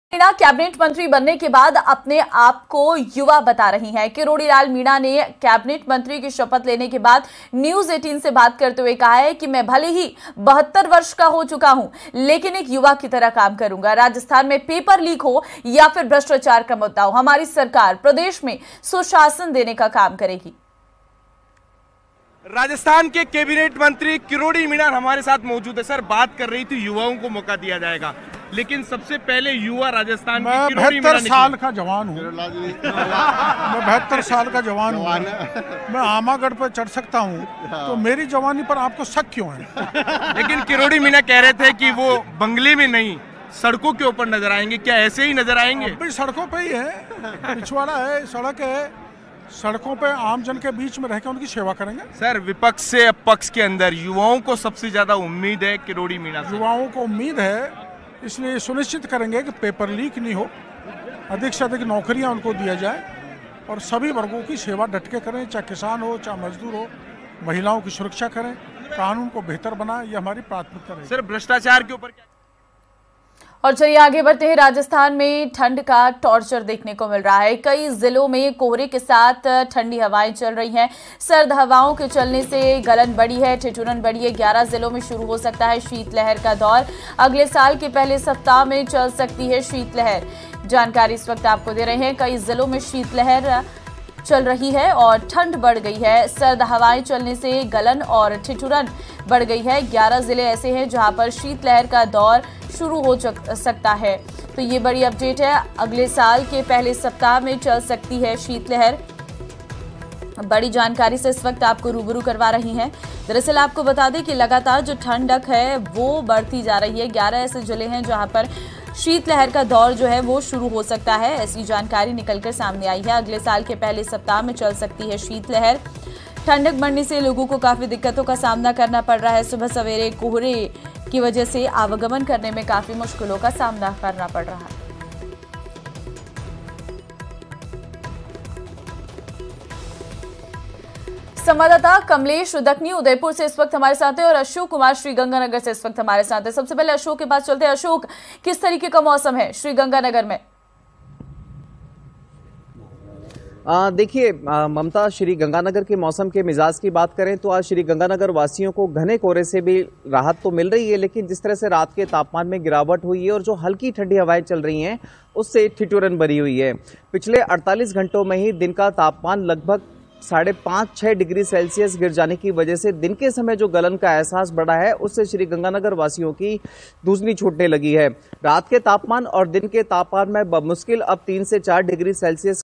Rajasthan Cabinet Expansion: मंत्री बनते ही उम्र को लेकर Kirodi Lal Meena का बड़ा बयान।Breaking News
शपथ लेने के बाद Kirodi Lal Meena ने News18 से बातचीत की. इस दौरान उन्होंने कहा कि वे 72 साल के जवान हैं.